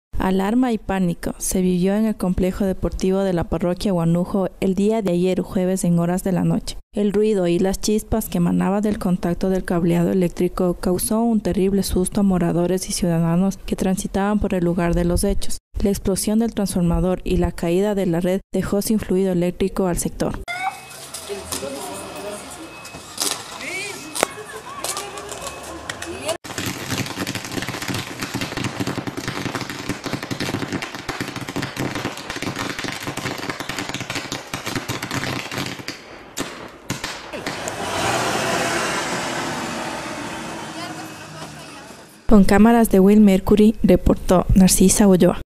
CORTÓ CIRCUITO EN CABLES DEL COMPLEJO DE GUANUJO.
Moradores de la plaza cordobés de la parroquia de Guanujo, se llevaron un  gran susto tras un corto circuito en las líneas del cableado eléctrico.